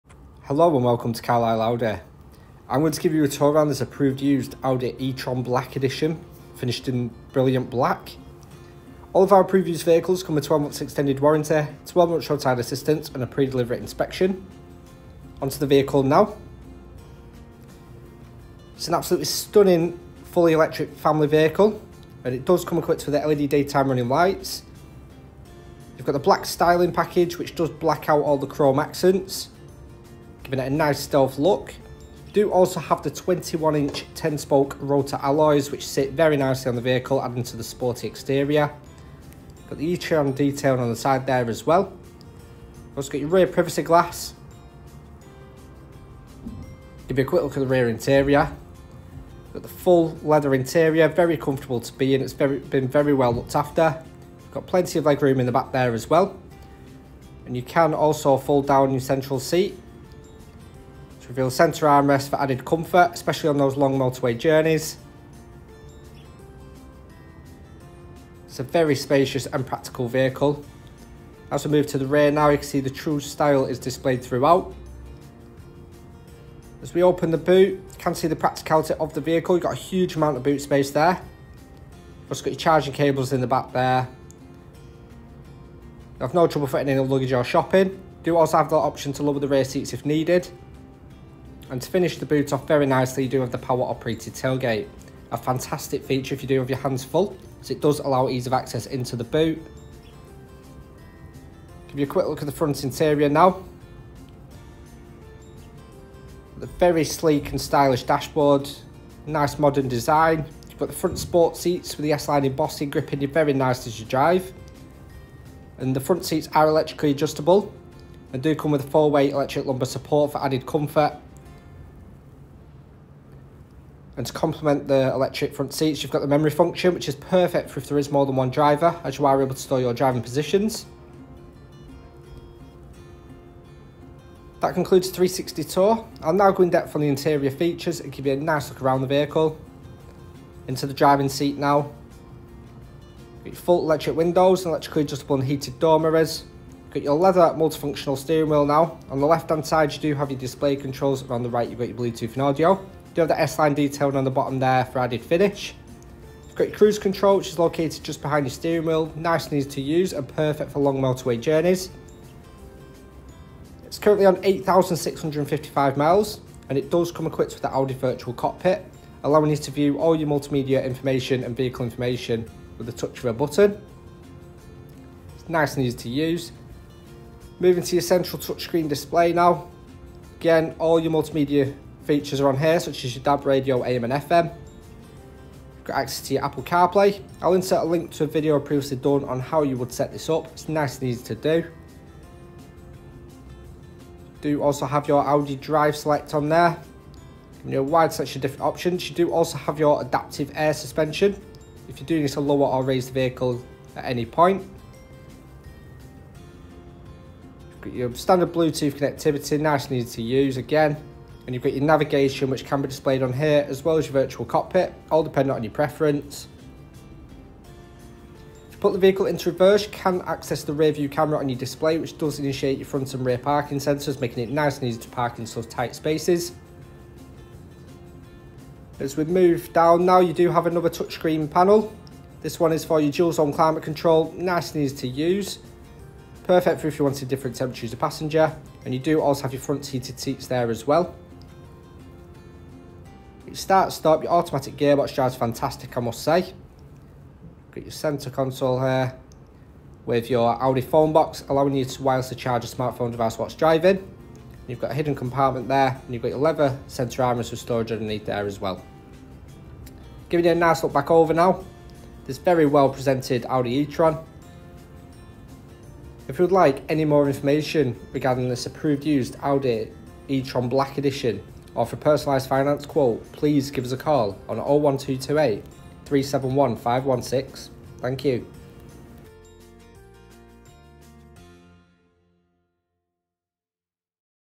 Approved Used Audi e-tron Black Edition | Carlisle Audi
Check out this Approved Used Audi e-tron Black Edition 50 quattro which is for sale at Carlisle Audi.

LM21DBX
8,370 miles
Electric
Automatic

Features include:

Navigation
Virtual Cockpit
Wireless Charging
Apple CarPlay

Introduction: 0:00
Exterior: 0:09
Rear Seats: 0:48
Boot: 1:16
Interior: 1:45
How to get in touch: 4:26